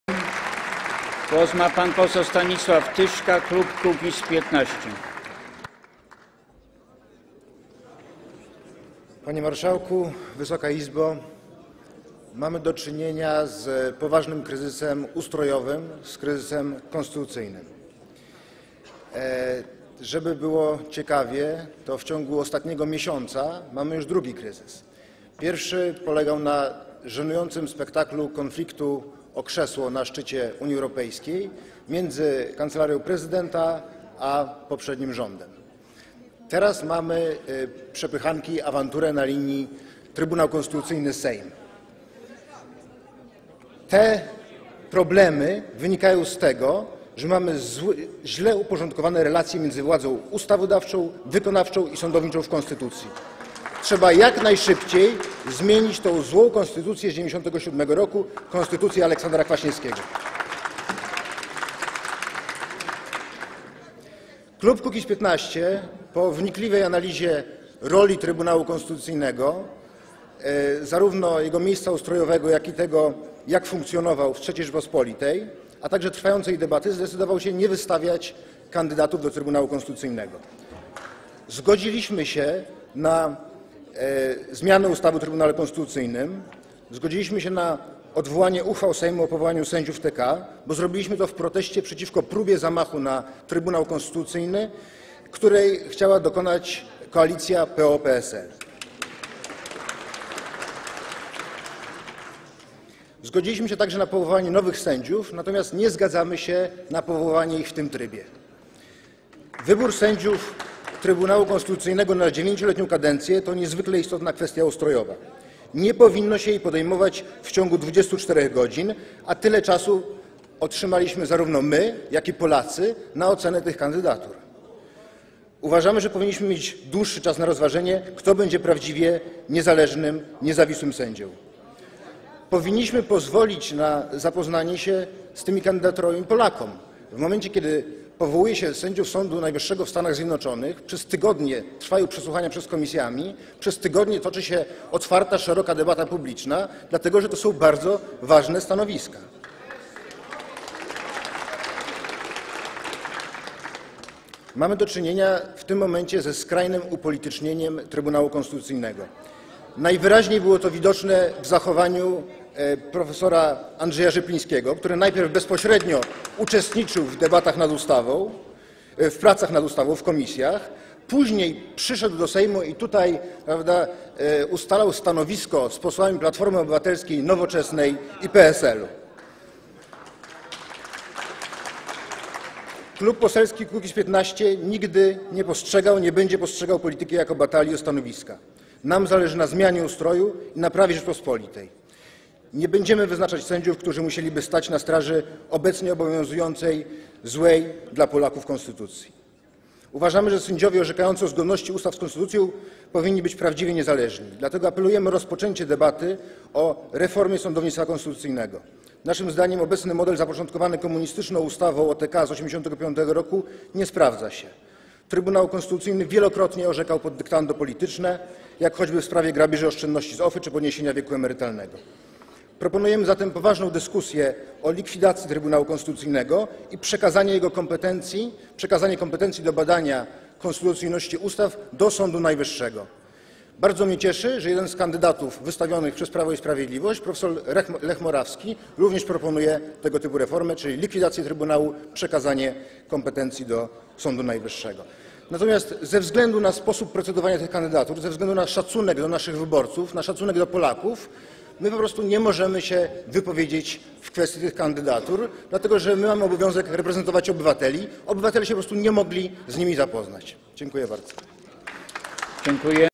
Mocne stanowisko Kukiz'15 ws. sędziów TK
Kliknij DZWONEK 🔔 aby być na bieżąco!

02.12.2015 r. - 3. posiedzenie Sejmu VIII kadencji (ws. wyboru pięciu nowych sędziów Trybunału Konstytucyjnego). – Konstytucja musi zostać jak najszybciej zmieniona. To z niej wynikają obecne problemy – powiedział poseł Stanisław Tyszka z Kukiz'15.

 Panie Marszałku! Wysoka Izbo! Mamy do czynienia z poważnym kryzysem ustrojowym, z kryzysem konstytucyjnym. Żeby było ciekawiej, w ciągu ostatniego miesiąca mamy już drugi kryzys. Pierwszy polegał na żenującym spektaklu - konflikcie o krzesło na szczycie Unii Europejskiej między Kancelarią Prezydenta a poprzednim rządem. Teraz mamy przepychanki, awanturę na linii Trybunał Konstytucyjny - Sejm.

    (Głos z sali: To nie ten czas!)

    Te problemy wynikają z tego, że mamy źle uporządkowane relacje między władzą ustawodawczą, wykonawczą i sądowniczą w konstytucji. (Oklaski)

    (Głos z sali: Aaa... Okej.)

    (Głos z sali: Aaa...)

    Trzeba jak najszybciej zmienić tę złą konstytucję z 1997 r., konstytucję Aleksandra Kwaśniewskiego. (Oklaski)

    (Poseł Ewa Kopacz: Nie, oczywiście, wszystko w jednych rękach, cała władza w jednych rękach.)

    Klub Kukiz’15 po wnikliwej analizie roli Trybunału Konstytucyjnego, zarówno jego miejsca ustrojowego, jak i tego, jak funkcjonował w III Rzeczypospolitej, a także trwającej debaty zdecydował się nie wystawiać kandydatów do Trybunału Konstytucyjnego. Zgodziliśmy się na zmianę ustawy o Trybunale Konstytucyjnym, zgodziliśmy się na odwołanie uchwał Sejmu o powołaniu sędziów Trybunału Konstytucyjnego, bo zrobiliśmy to w proteście przeciwko próbie zamachu na Trybunał Konstytucyjny, której chciała dokonać koalicja PO-PSL. (Oklaski)

    (Poseł Magdalena Kochan: Teraz dopiero nie ma zamachu.)

    Zgodziliśmy się także na powoływanie nowych sędziów, natomiast nie zgadzamy się na powołanie ich w tym trybie. (Oklaski)

    Wybór sędziów Trybunału Konstytucyjnego na 9-letnią kadencję to niezwykle istotna kwestia ustrojowa. Nie powinno się jej podejmować w ciągu 24 godzin, a tyle czasu otrzymaliśmy zarówno my, jak i Polacy na ocenę tych kandydatur.

    (Poseł Sławomir Nitras: Nie dali wam.)

    Uważamy, że powinniśmy mieć dłuższy czas na rozważenie, kto będzie prawdziwie niezależnym, niezawisłym sędzią. Powinniśmy pozwolić na zapoznanie się z tymi kandydaturami Polakom. W momencie kiedy powołuje się sędziów Sądu Najwyższego w Stanach Zjednoczonych, przez tygodnie trwają przesłuchania przed komisjami, przez tygodnie toczy się otwarta, szeroka debata publiczna, dlatego że to są bardzo ważne stanowiska. (Oklaski)

    (Poseł Rafał Grupiński: Nie w PiS-ie!)

    (Poseł Magdalena Kochan: O, szybko wam przyszło.)

    Mamy do czynienia w tym momencie ze skrajnym upolitycznieniem Trybunału Konstytucyjnego. Najwyraźniej było to widoczne w zachowaniu prof. Andrzeja Rzeplińskiego, który najpierw bezpośrednio uczestniczył w pracach nad ustawą, w komisjach, później przyszedł do Sejmu i tutaj, prawda, ustalał stanowisko z posłami Platformy Obywatelskiej, Nowoczesnej i PSL. (Oklaski)

    (Poseł Ewa Kopacz: Co ty gadasz?)

    (Poseł Sławomir Nitras: Ale co ty gadasz?)

    Klub Poselski Kukiz’15 nigdy nie postrzegał, nie będzie postrzegał polityki jako batalii o stanowiska. Nam zależy na zmianie ustroju i naprawie Rzeczypospolitej. Nie będziemy wyznaczać sędziów, którzy musieliby stać na straży obecnie obowiązującej, złej dla Polaków konstytucji. Uważamy, że sędziowie orzekający o zgodności ustaw z konstytucją powinni być prawdziwie niezależni. Dlatego apelujemy o rozpoczęcie debaty o reformie sądownictwa konstytucyjnego.

    Naszym zdaniem obecny model, zapoczątkowany komunistyczną ustawą o TK z 1985 r., nie sprawdza się. Trybunał Konstytucyjny wielokrotnie orzekał pod dyktando polityczne, jak choćby w sprawie grabieży oszczędności z OFE czy podniesienia wieku emerytalnego. Proponujemy zatem poważną dyskusję o likwidacji Trybunału Konstytucyjnego i przekazanie jego kompetencji, kompetencji do badania konstytucyjności ustaw, do Sądu Najwyższego. Bardzo mnie cieszy, że jeden z kandydatów wystawionych przez Prawo i Sprawiedliwość, prof. Lech Morawski, również proponuje tego typu reformę, czyli likwidację trybunału i przekazanie kompetencji do Sądu Najwyższego.

    Natomiast ze względu na sposób procedowania tych kandydatur, ze względu na szacunek dla naszych wyborców, na szacunek dla Polaków, po prostu nie możemy się wypowiedzieć w kwestii tych kandydatur, dlatego że mamy obowiązek reprezentować obywateli. Obywatele się po prostu nie mogli z nimi zapoznać. Dziękuję bardzo.